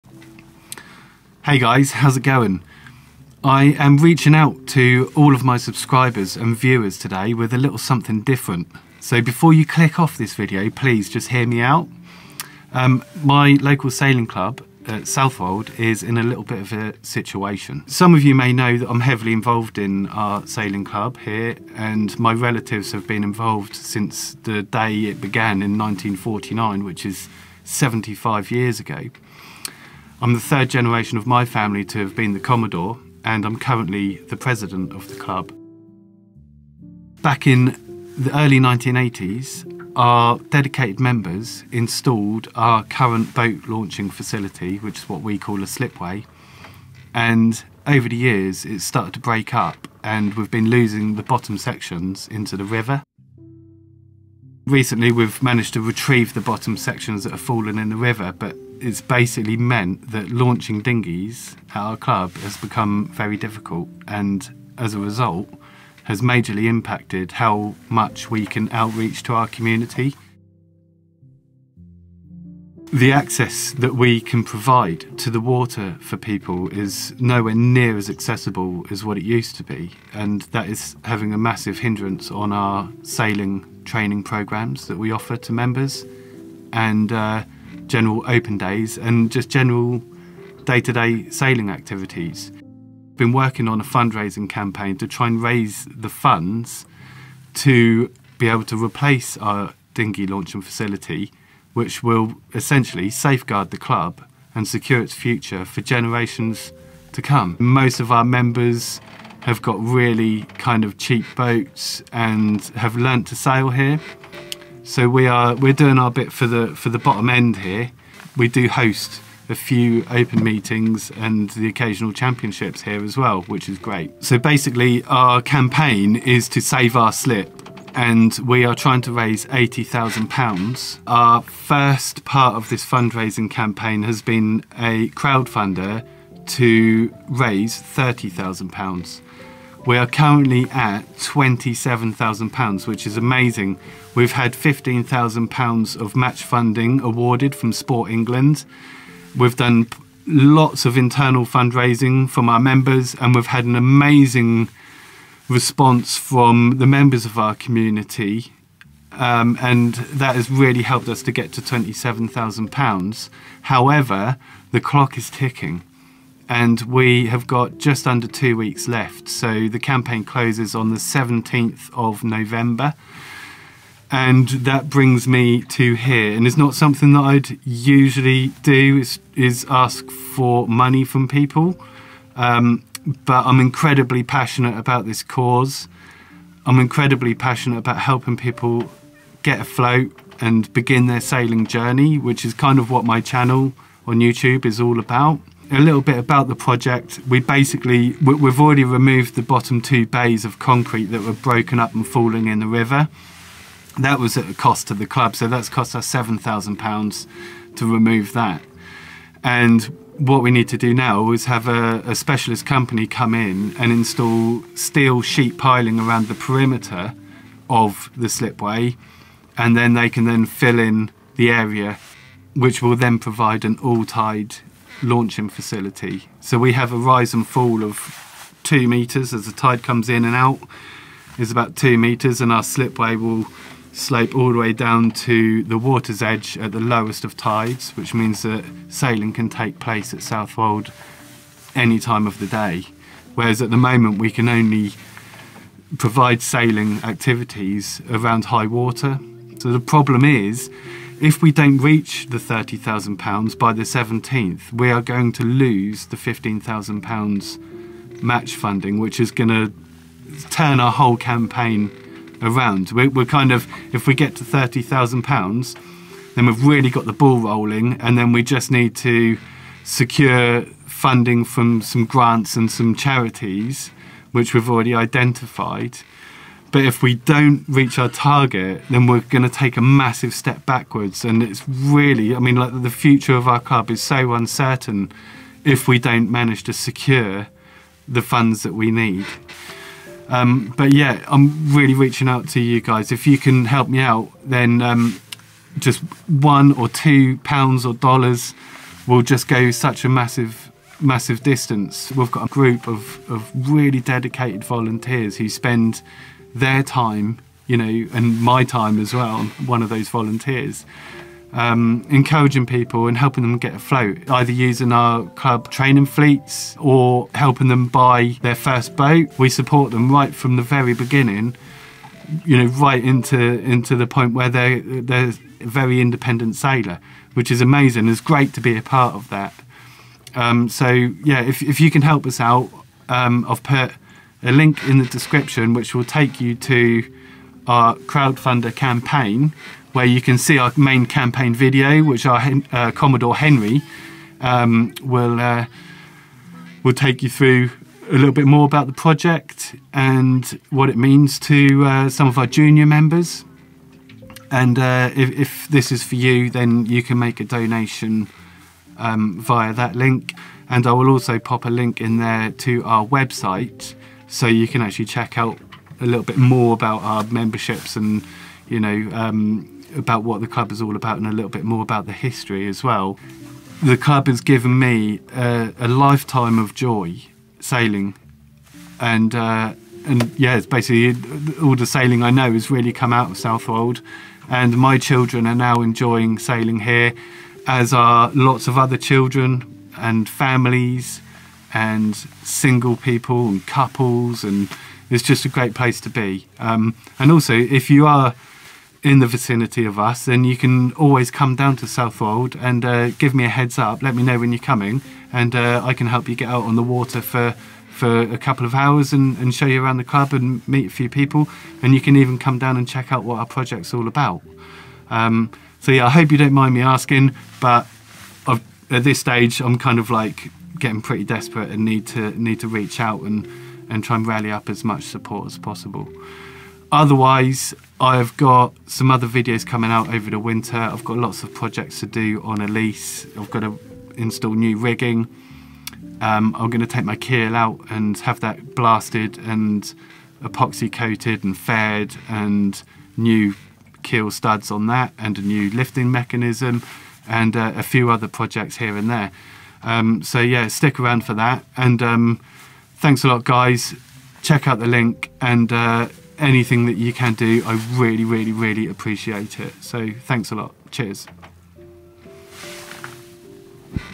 Please help me secure the future of our local sailing club!
I'm reaching out for a little support for a great cause. My local sailing club's future is uncertain and the clock is ticking. I'm currently running a fundraising campaign to replace the launching facility at Southwold Sailing Club. We have 2 weeks left (17th November it closes) to raise the remaining £3000 to reach our target of £30,000. If we don't reach this target we will lose £15,000 match funding and our entire project will be doomed! I'm getting a little desperate and although I wouldn't normally ask people for money, I feel I need to do everything I can to help secure the club for generations to come. The campaign is all about securing our clubs future and to keep the door open to sailing at Southwold. Our grass roots club supports members of our community to begin their sailing journey, from learning to sail, buying their first boat to eventually being a self sufficient and confident sailor.

Check out our campaign here, share amongst friends and please consider making a donation.